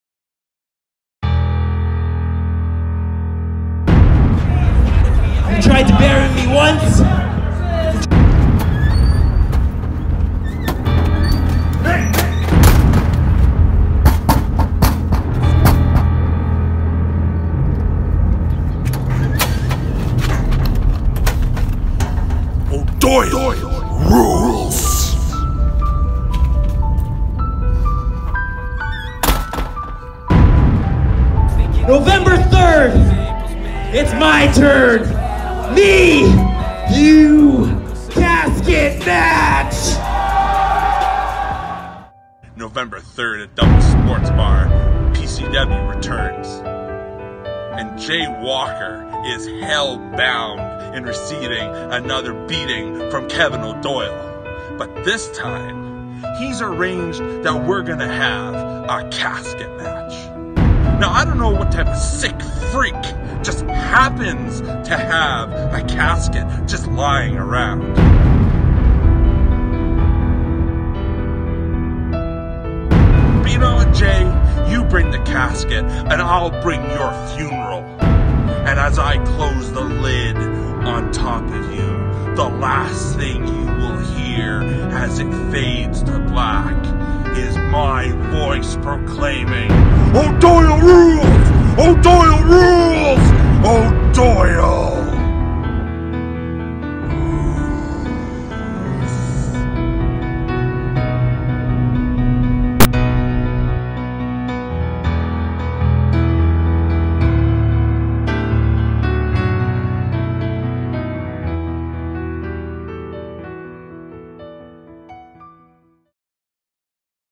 Casket Match Hype!
Jay Walker vs Kevin O'Doyle - CASKET MATCH - See it LIVE, Saturday Nov. 3rd 2018 at PCW's "Hellbound"!